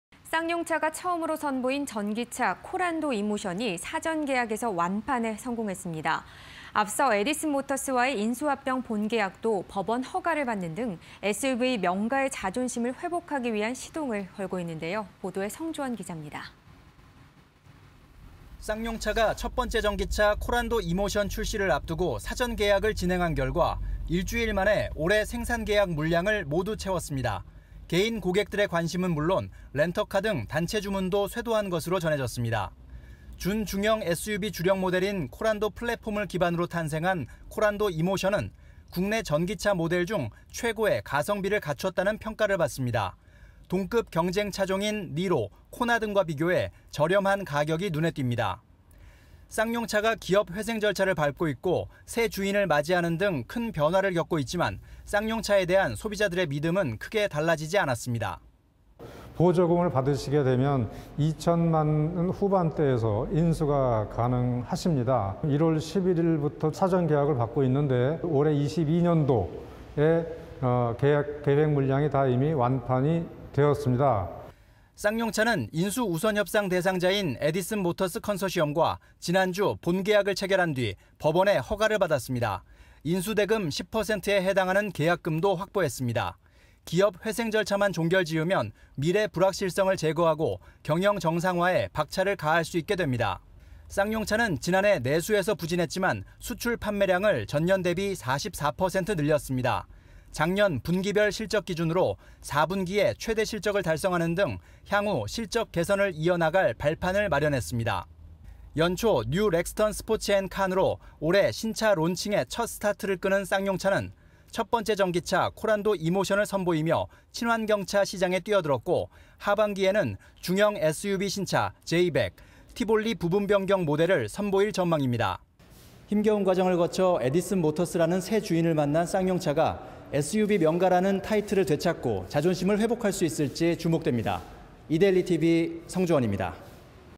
첫 전기차 사전계약 '완판'… 새주인 맞는 쌍용차, 봄날 오나 (20220119)
# ‘코란도 이모션’ 사전계약 일주일만에 ‘완판’
- 보조금 적용시 2000만원대 후반… 가격경쟁력 돋보여
- 쌍용자동차 브랜드에 대한 소비자 신뢰 ‘증명’
- M&A 본계약 법원 허가… 기업 회생절차만 남아
- 하반기 중형 SUV 신차 ‘J100’ 등 출시 예정